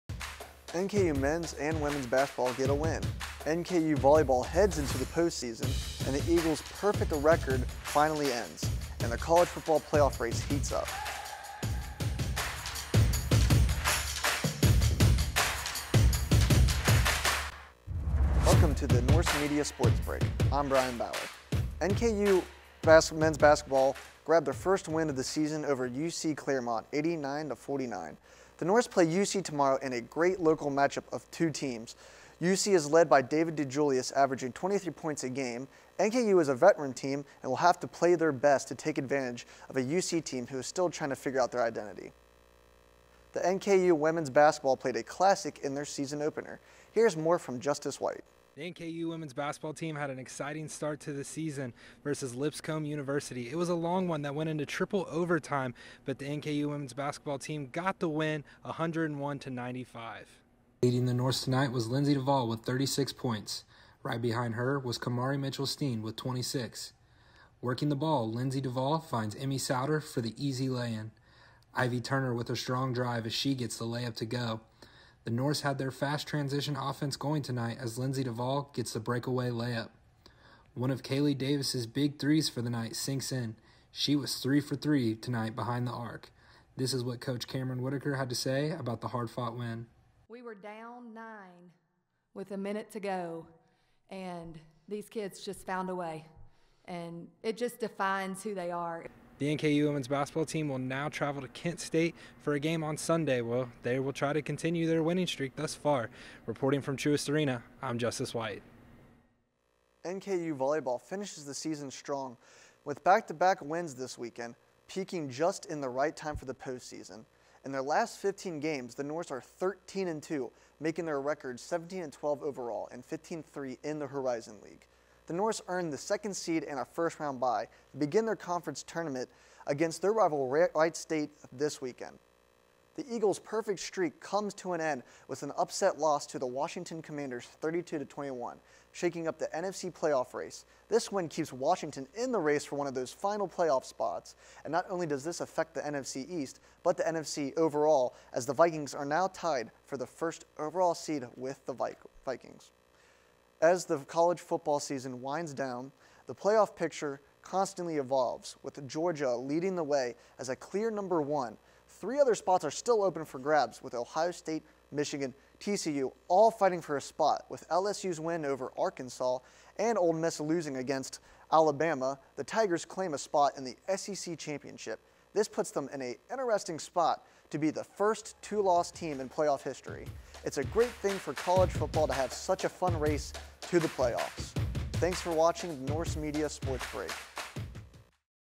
Norse Media Sports Break Ep. 1
Bryan Bauer brings you NKU Athletics' updates, this past week in the NFL, and the College Football Playoff picture.

This is an on-going producing project in Wes Akers Electronic Media & Broadcasting Sports Production class.